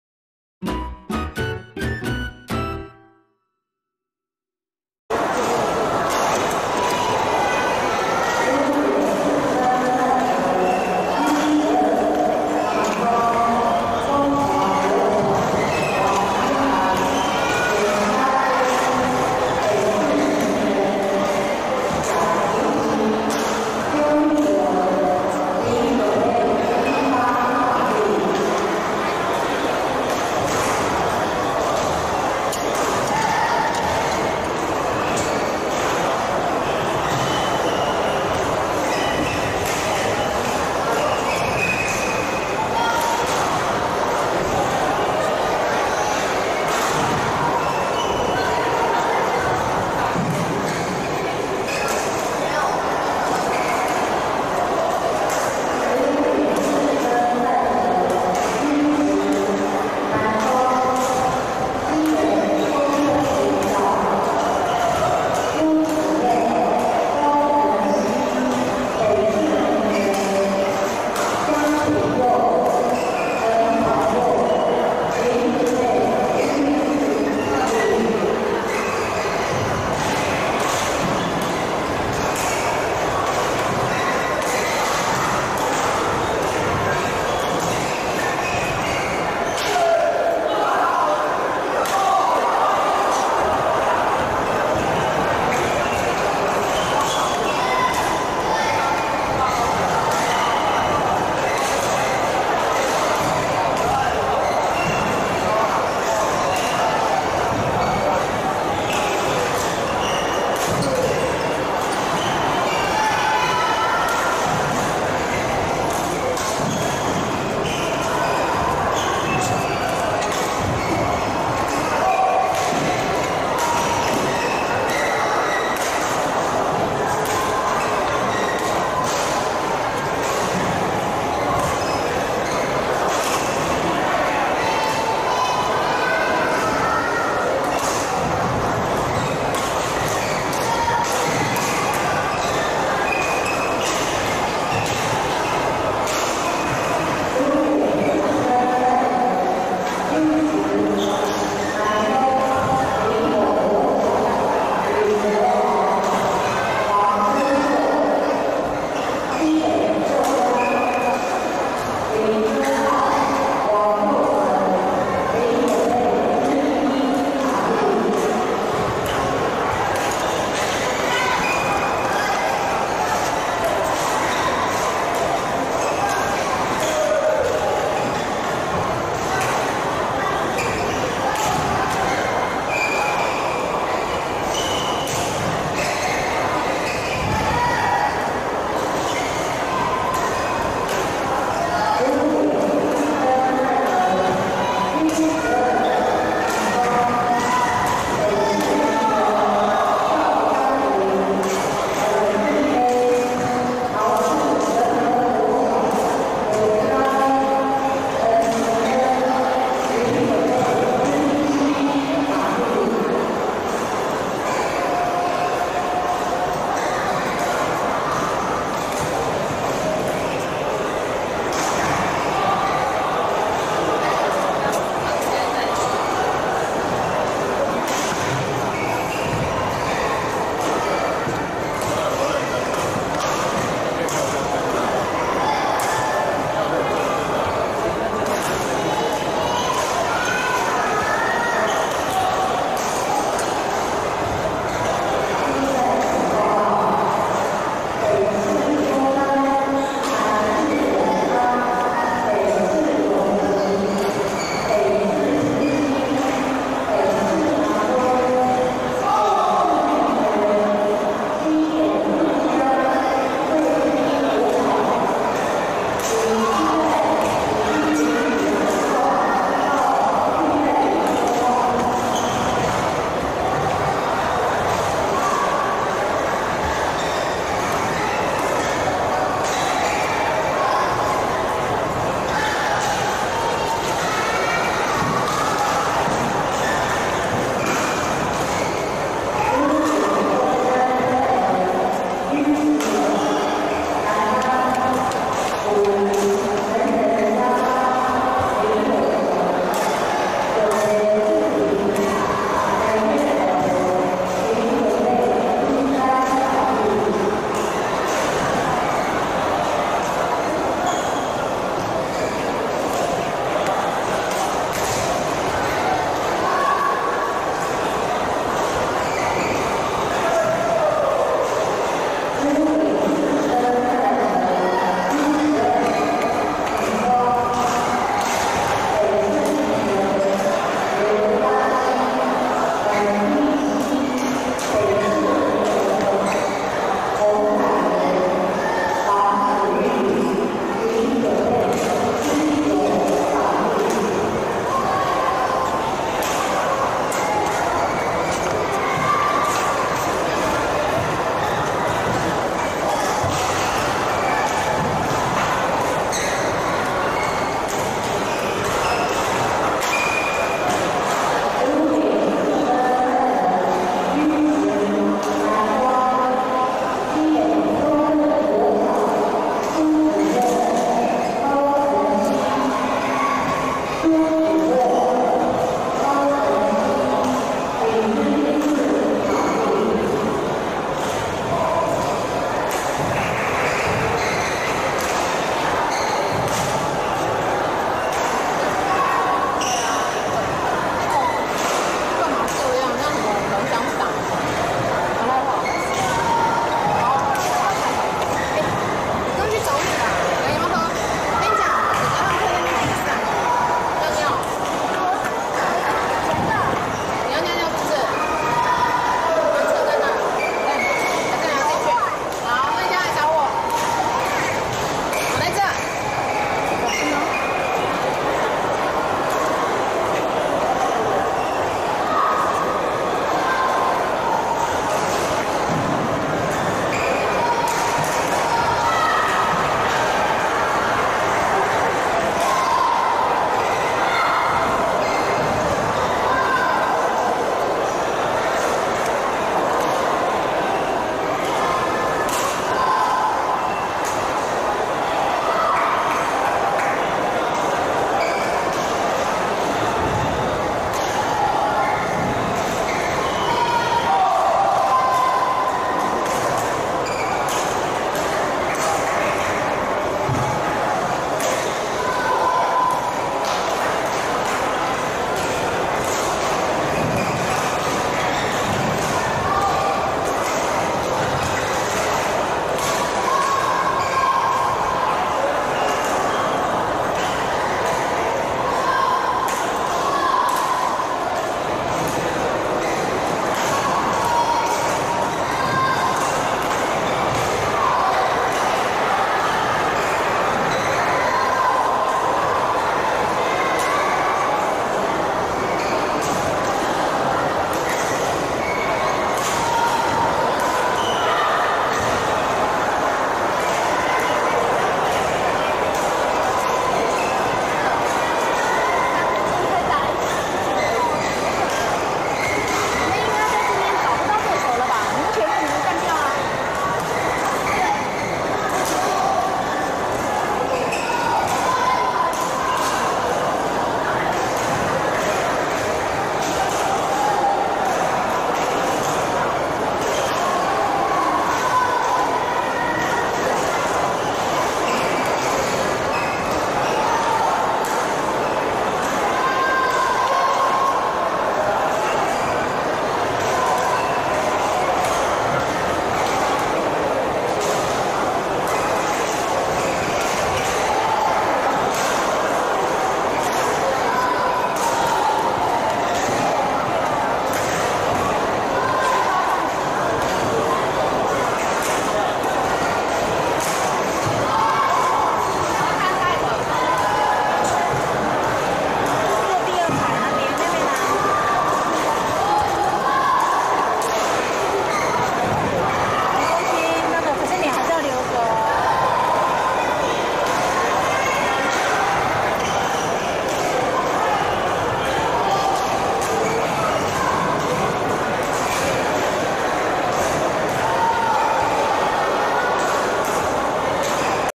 (直播)U10MD許睿紘/詹景荃(北市民權)vs江政璋/簡子恆(桃市仁和)(2020會長盃羽球賽)
(直播)U10MD許睿紘/詹景荃(北市民權)31vs江政璋/簡子恆(桃市仁和)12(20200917會長盃羽球賽)